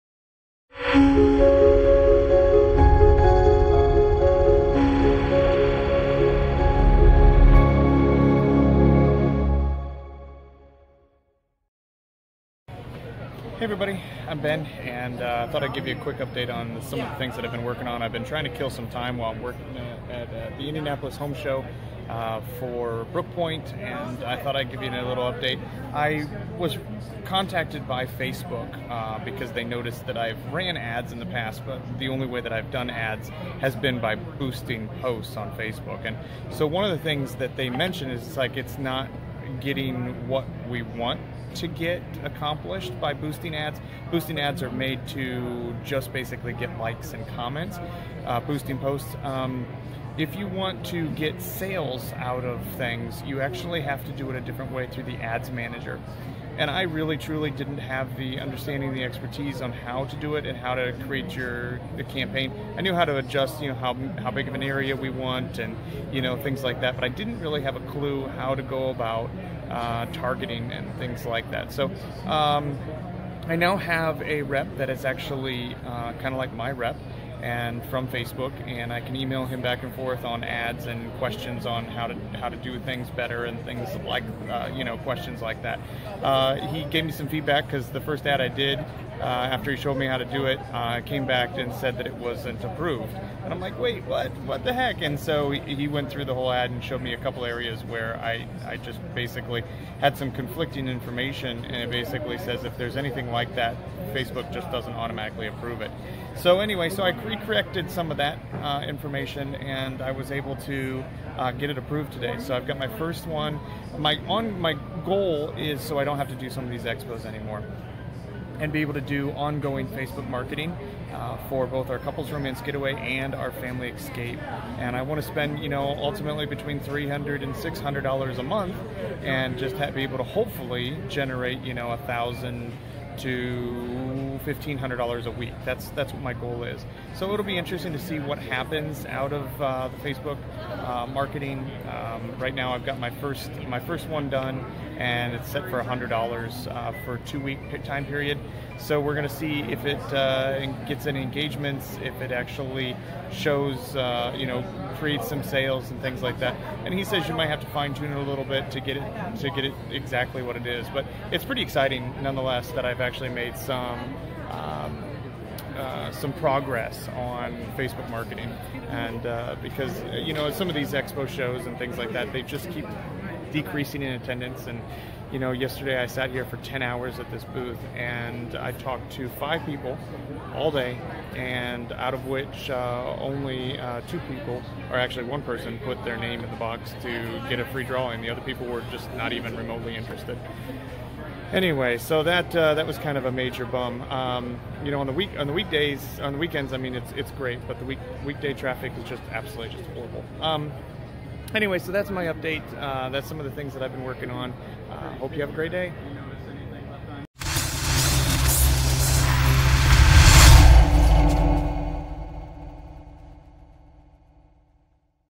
2020.01.22 - Learning how to market on Facebook to decrease marketing at expos
Ben traveling around to home shows has been a profitable adventure over the last several years.  Attendance has been dropping over the last couple of years and I believe the show attendance will not be increasing much in the coming years.  If I can begin advertising and promoting our products over the internet at a cost effective method then that is the direction I need to go.  I have a test going for the next 2 weeks to see if it produces any sales for us.